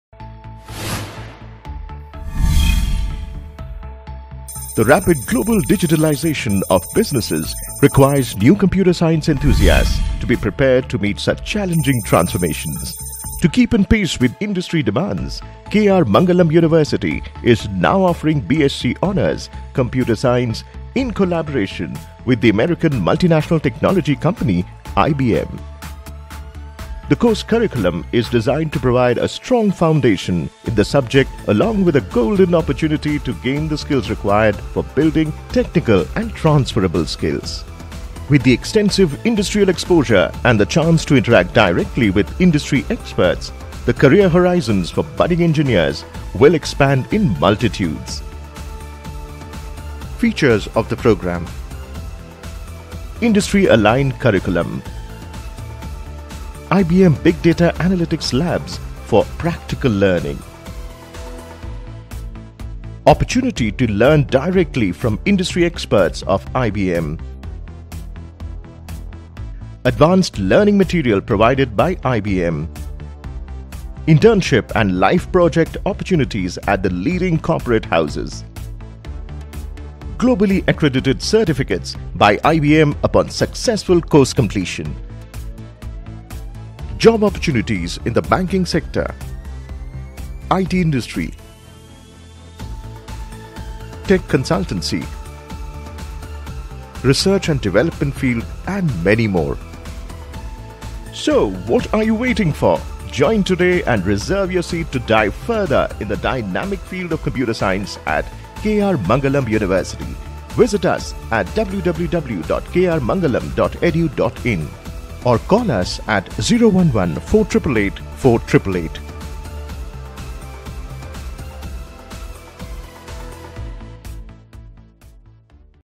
Pursue Bachelor of Science in Computer Science with IBM at K.R. Mangalam University | B.SC CS
B.Sc.(Hons.) - Computer Science with Academic Support of IBM programme at KR Mangalam University is a three-year course that is offered in Collaboration with the IBM Corporation. This collaboration enables students to become industry-ready and prepared to cope up with challenging demands of the post-pandemic era. Students that are enrolled in this course are taught skills to build software applications and make extensive use of Big Data like Hadoop.

School of Engineering and Technology (SOET) at KRMU has designed this course with the growing demands of the Information and Communications Technology (ICT) industry. ICT industry is quite vast and lucrative. KRMU facilitates its students with plentiful practical training lessons. The School also gives them the confidence to solve professional problems and apply their theoretical knowledge to real-life situations. Students are given multiple lessons on the processes involved in  various stages of Software Development. Apart from the regular coursework, students will also be provided with various certification courses.

Apply to our programs at admissions.krmangalam.edu.in
           or